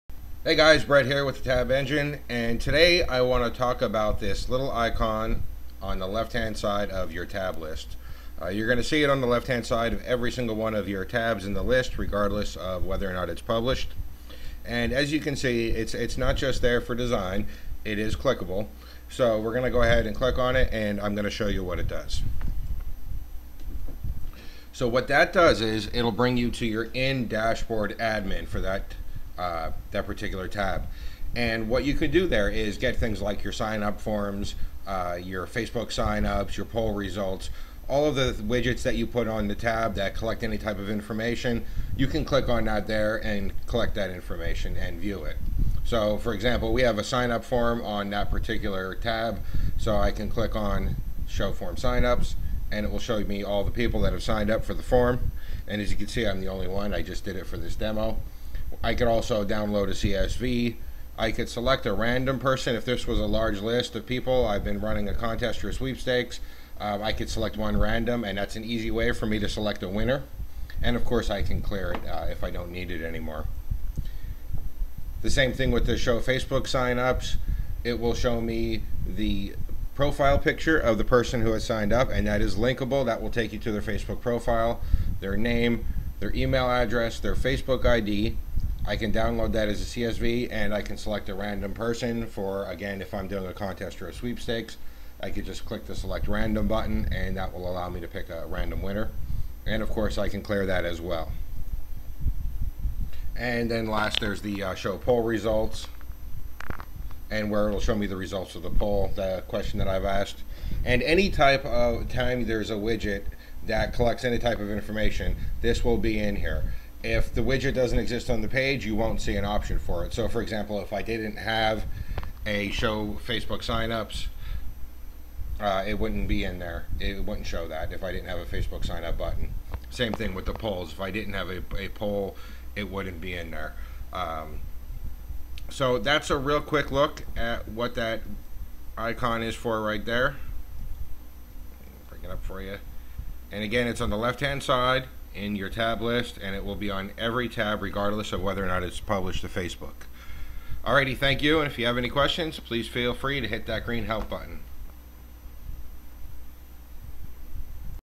Tab Engine, Gear icon
How to access a tabs admin area from inside the Tab Engine dashboard.